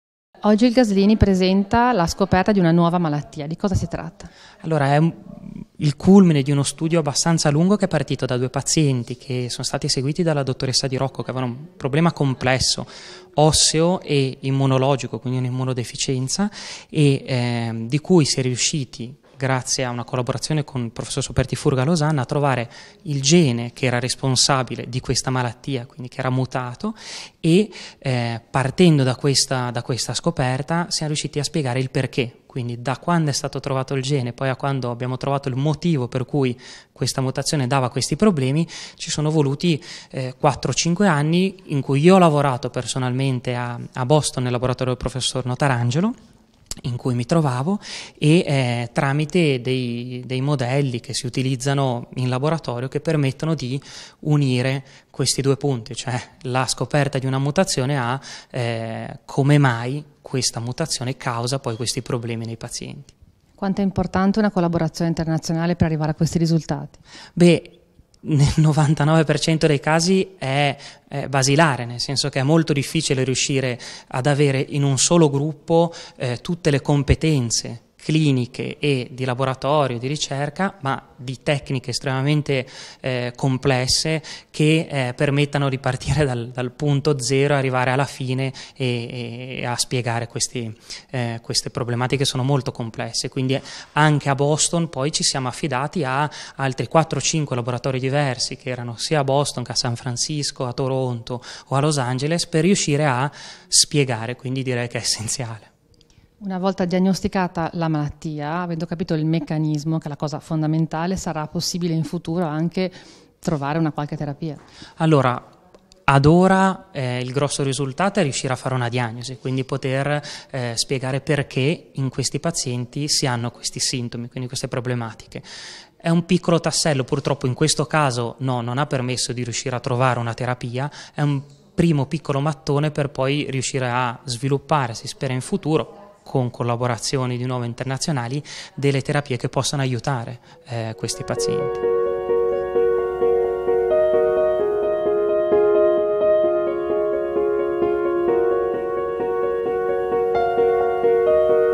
GENE EXTL3 ALL'ORIGINE DI MALFORMAZIONE OSSEA E IMMUNODEFICENZA
Importante risultato dell’Istituto Giannina Gaslini di Genova che, in collaborazione con le Università di Harvard e Losanna, ha dimostrato che mutazioni del gene EXTL3 sono all’origine di una nuova sindrome che causa displasia ossea e immunodeficienza. Lo studio ha richiesto un lungo lavoro di indagine compiuto in gran parte grazie alla perseveranza di un giovane ricercatore dell’Istituto Gaslini, il dottor Stefano Volpi.